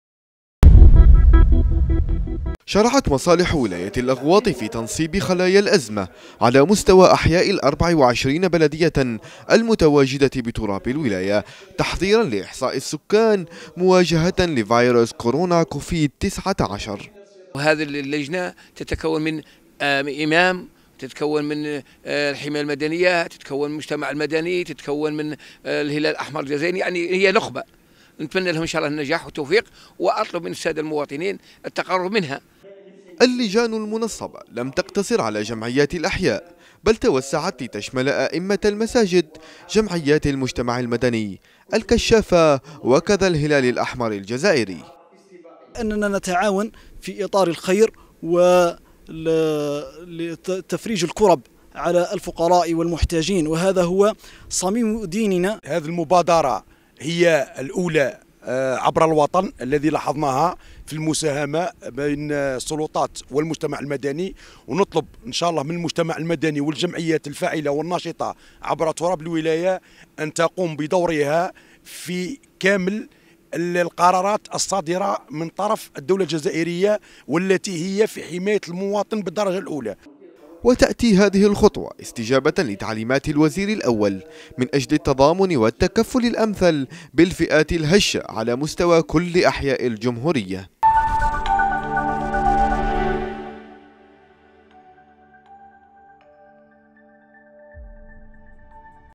تنصيب خلايا للأزمة بالأغواط لمواجهة وباء كورونا
الأغواط (وأج) - تم تنصيب خلايا للأزمة ببلديات الولاية من أجل إحصاء السكان بهدف وضع خطة عمل لمواجهة تفشي فيروس كورونا بالمنطقة.